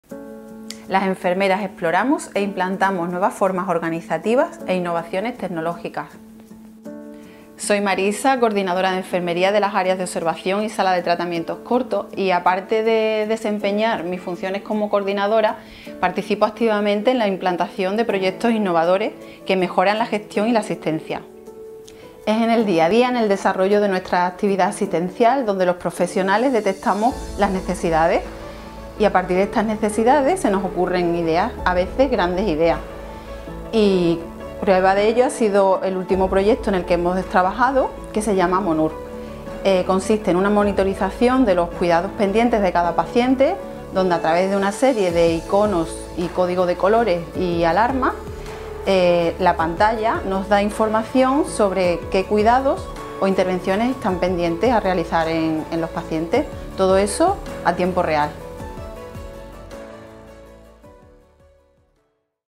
DíaInternacionaldelaEnfermería - Marisa, enfermera coordinadora de observación
Las enfermeras exploran e implantan nuevas fórmulas organizativas e innovaciones tecnológicas. Marisa Quintero Solís, enfermera coordinadora de observación y tratamientos cortos del Hospital San Juan de Dios del Aljarafe.